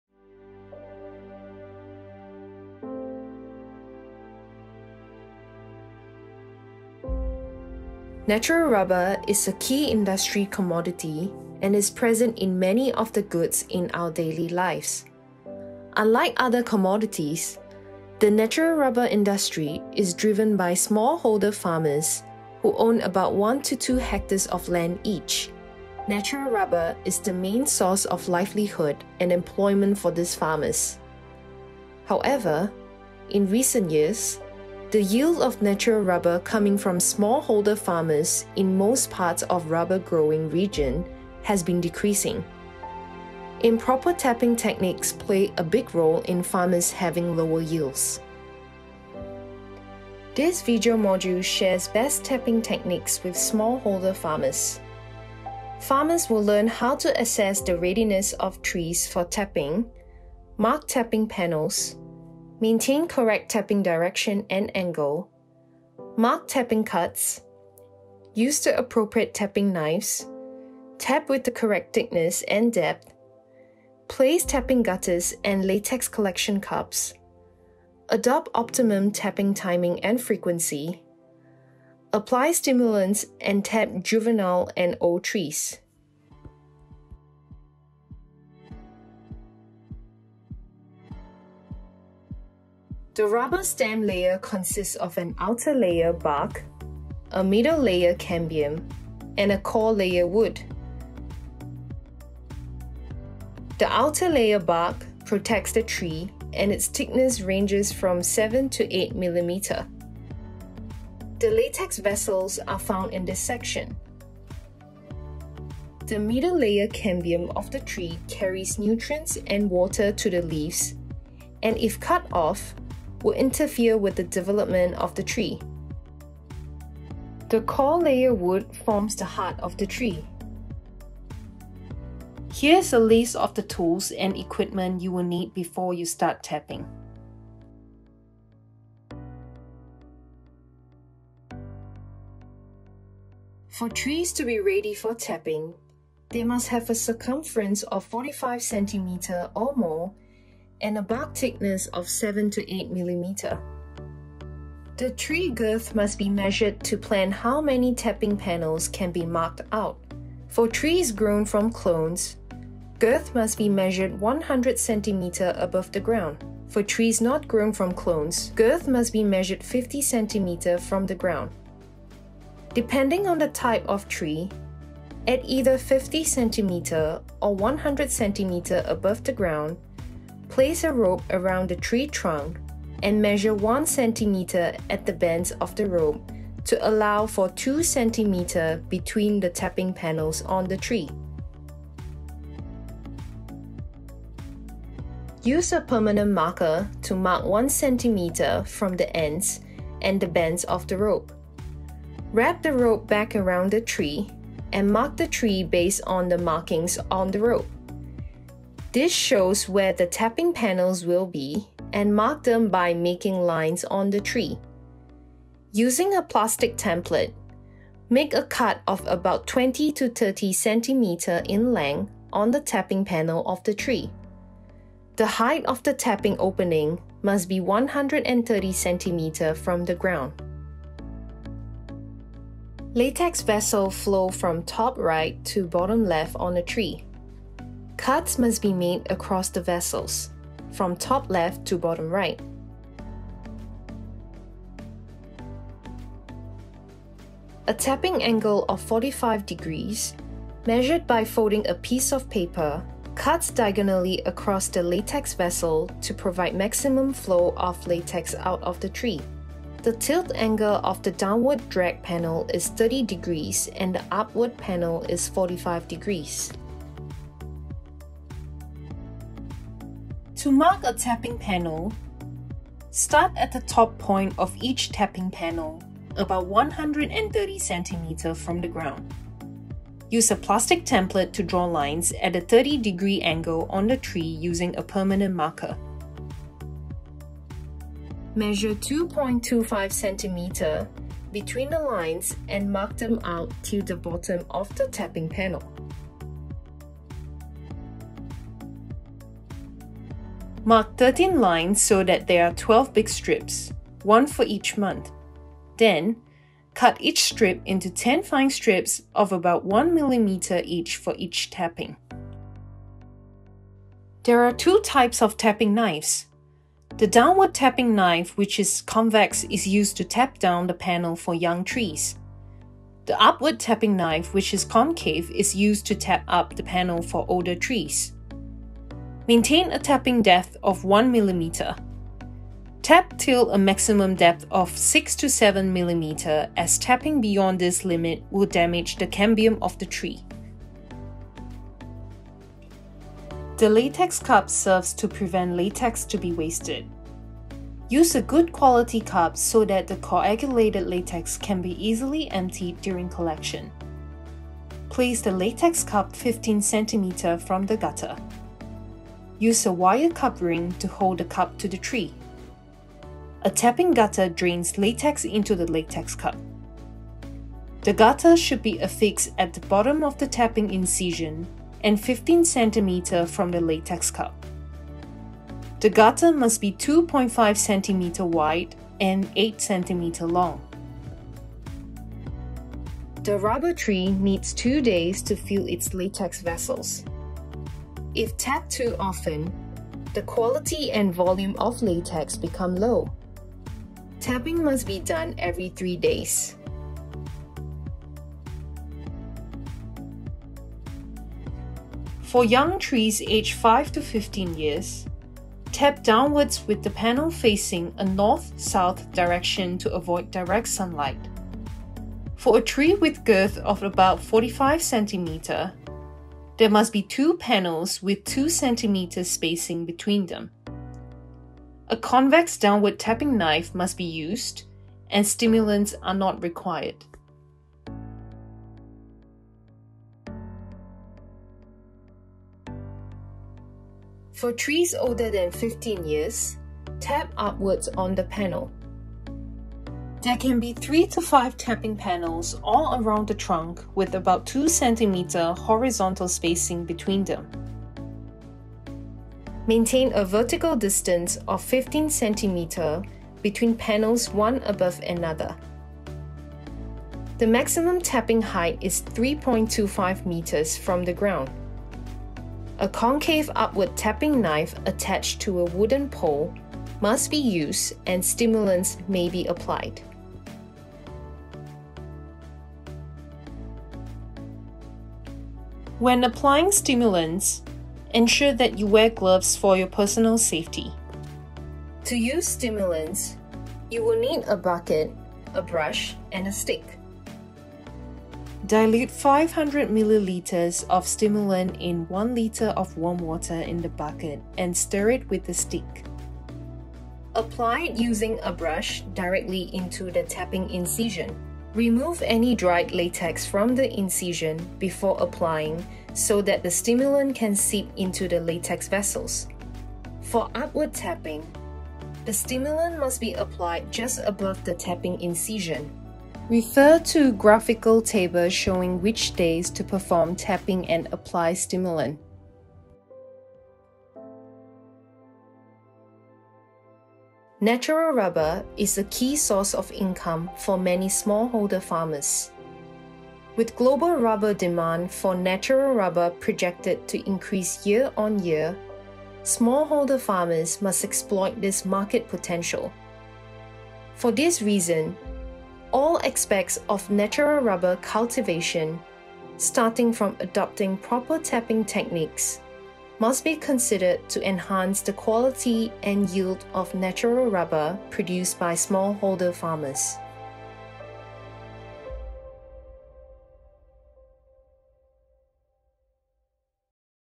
Agridence Rubber Grow Module 1 - Rubber tapping
This is the first of five modules developed by Agridence and SNV to digitalize Good Agricultural Practices for natural rubber smallholder farmers.

The module covers tools and techniques for tapping rubber trees.

Video Outline
0:00 Introduction to rubber tapping
1:32 Interior of rubber tree and tapping tools used
2:23 Preparation for tapping rubber tree
5:14 How to tap rubber tree and latex cup setup
6:41 Tapping of young and old rubber trees
7:55 How to use stimulant